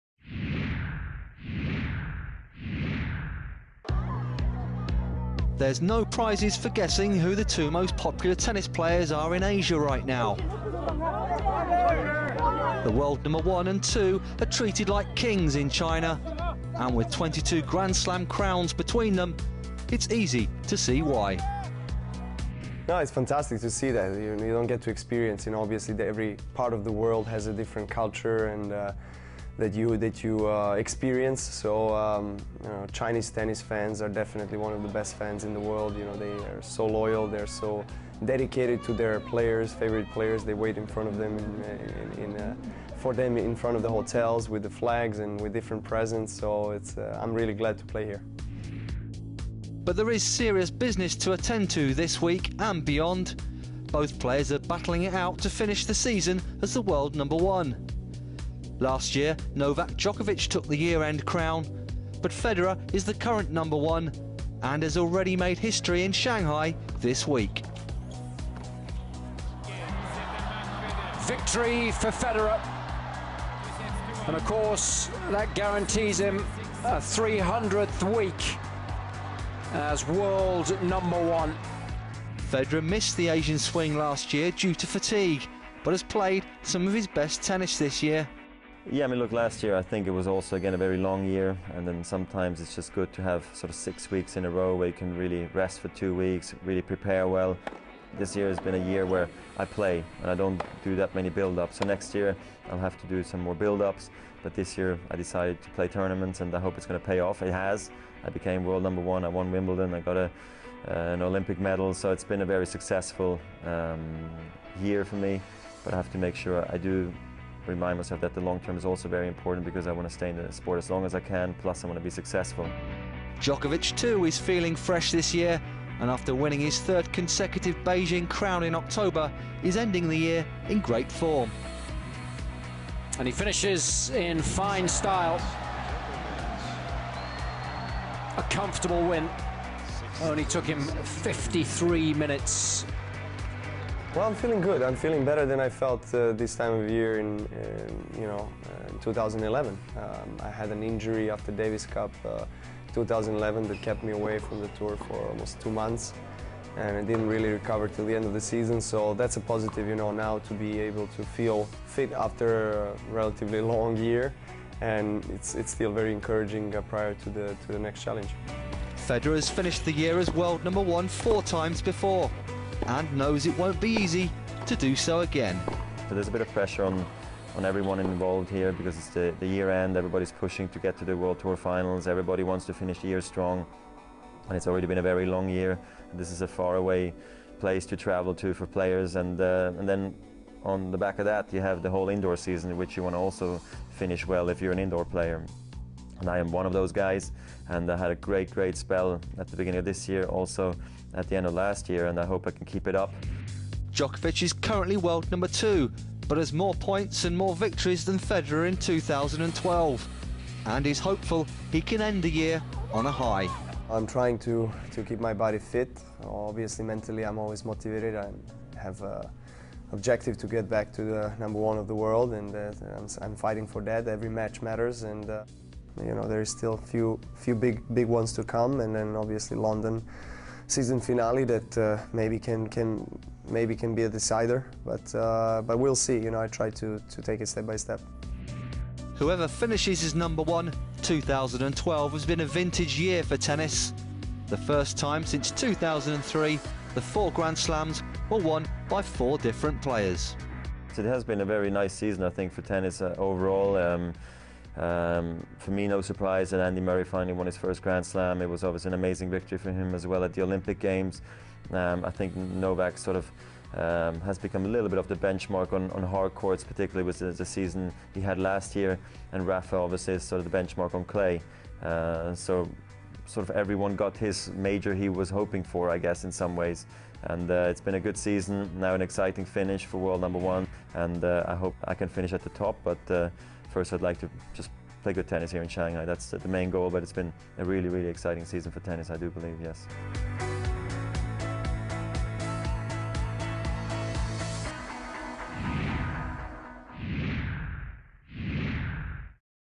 Race For No. 1 Heats Up In Shanghai
Take a closer look at the contest between Roger Federer and Novak Djokovic to finish year-end World No. 1 in the South African Airways ATP Rankings.